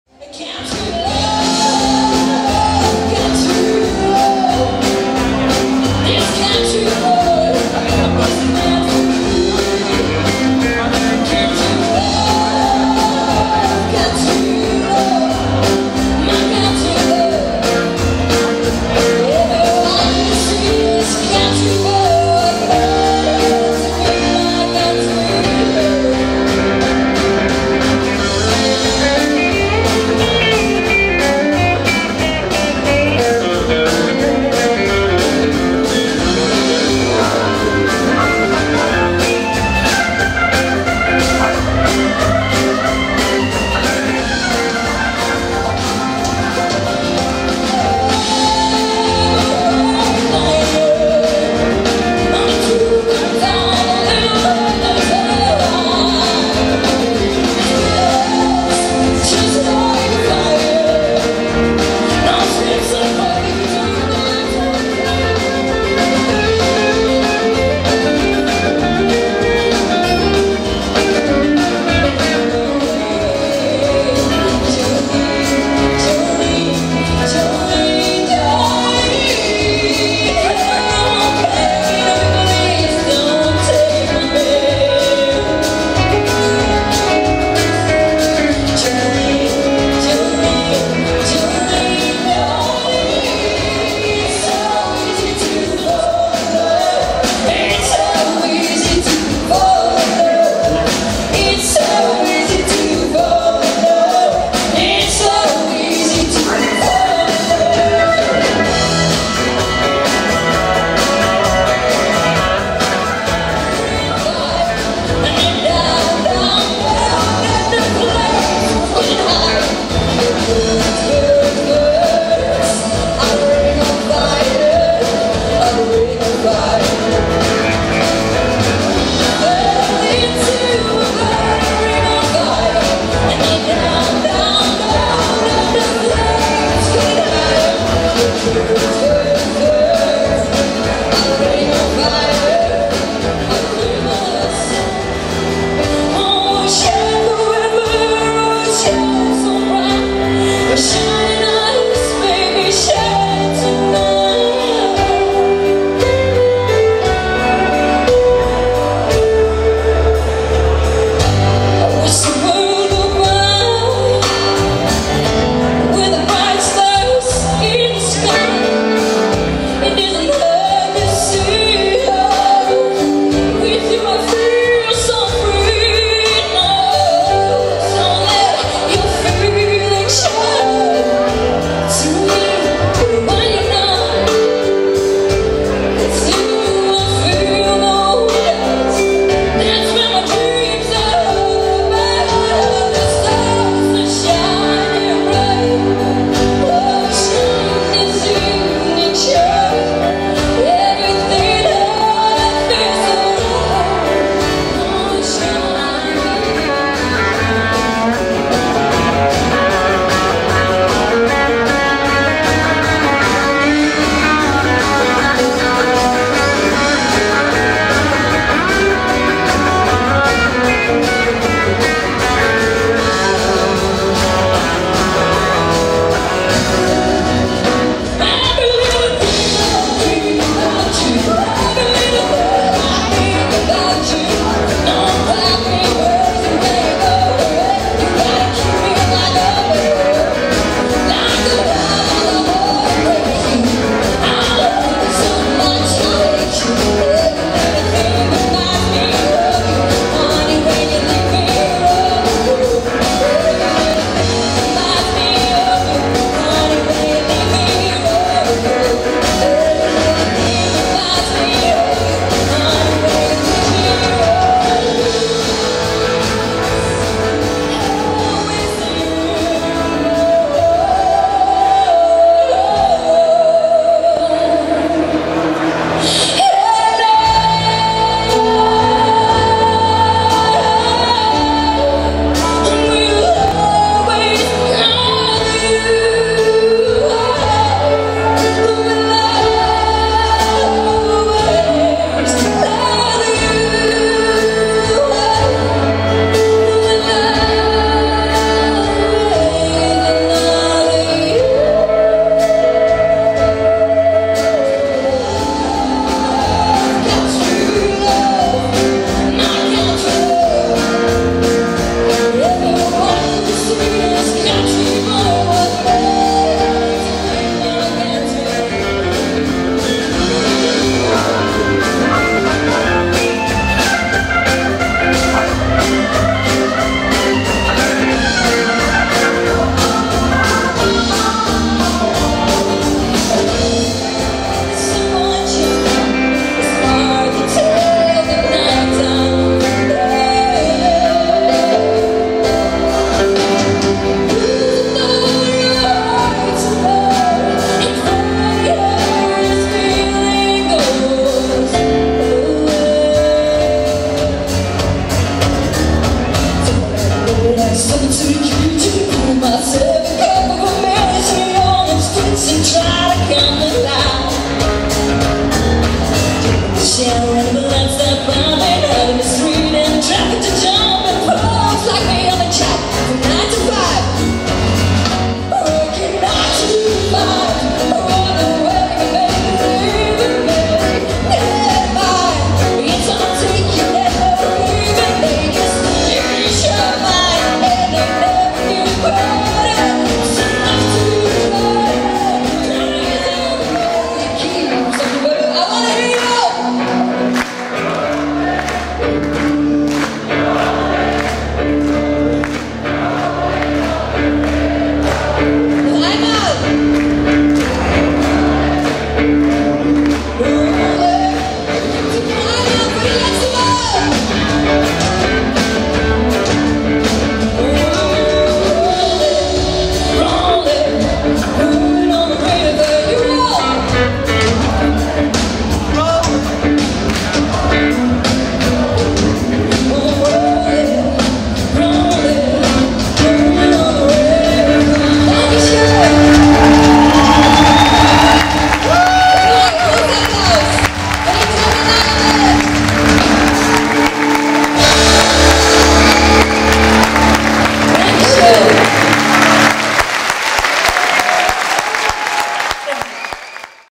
Laura van den Elzen 17 years & Jambalaya Band live in BERLIN - Country Music Meeting - DSDS 2016
Laura van den Elzen ( NL) & Jambalaya Band (DK) great performance in BERLIN - Country Music Meeting